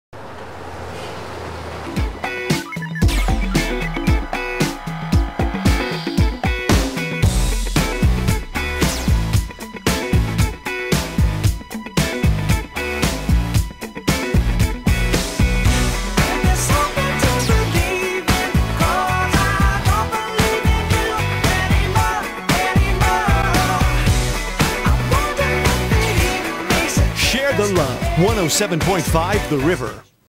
Share The Love
Our submission for the River Tube contest for 107.5 the River.  Featuring Mt. Juliet High School students David Altizer, Gavin Dowlen, Michael Longstreth, and Jim McVey.  Special effects and editing were done by David Altizer.  David is this year's Nicktoons "Show Off Your Show" Animation contest grand prize winner.